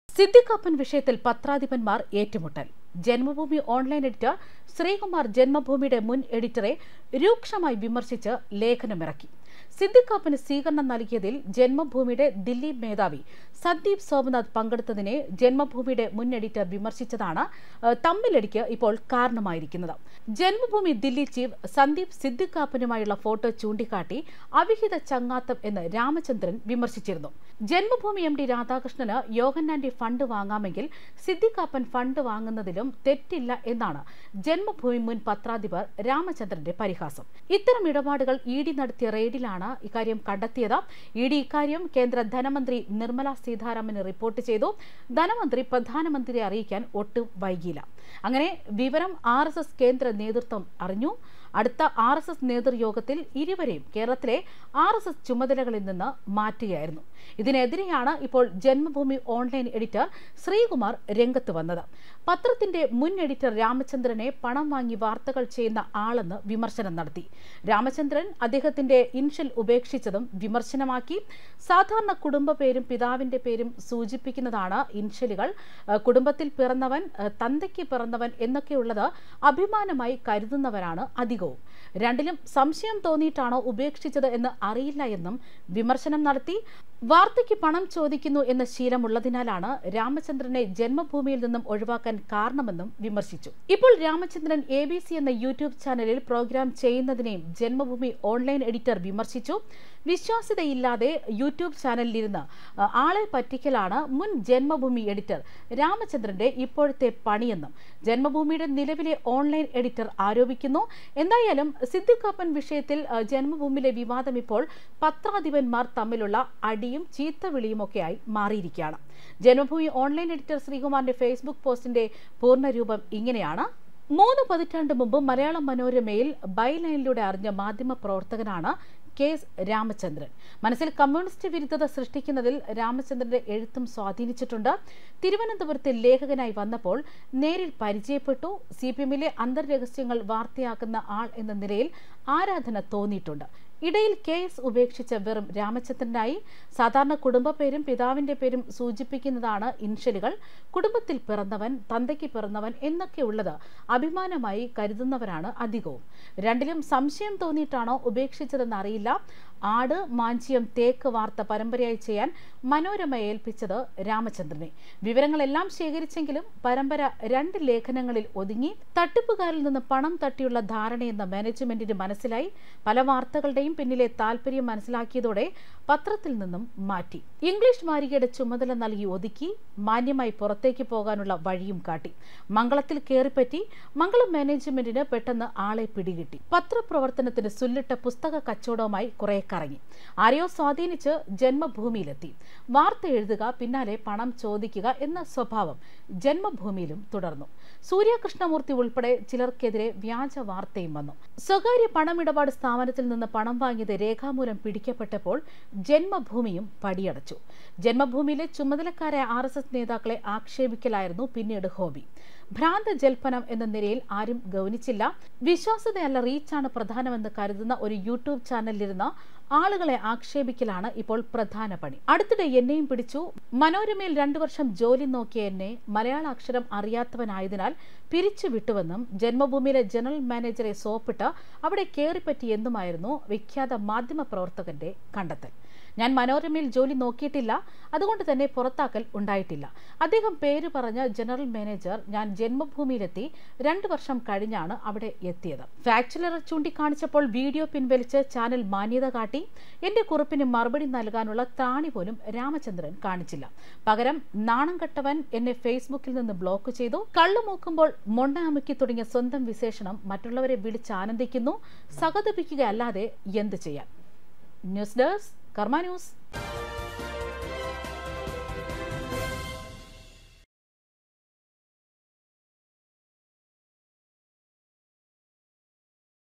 ജന്മഭൂമി | തന്തക്ക് വിളി ,പണം വാങ്ങൽ പത്രാധിപന്മാർ ഏറ്റുമുട്ടൽ Editors Debate
സിദ്ദിഖ് കാപ്പൻ കീറാമുട്ടിയായി, വിഷയത്തിൽ ജന്മഭൂമി മുൻ പത്രാധിപരും നിലവിലെ പത്രാധിപരും ഏറ്റുമുട്ടി,തന്തക്ക് വിളി ,പണം വാങ്ങൽ,കള്ള് മൂക്കുമ്പോള്‍. ‘മൊണ്ണ’, ‘അമുക്കി’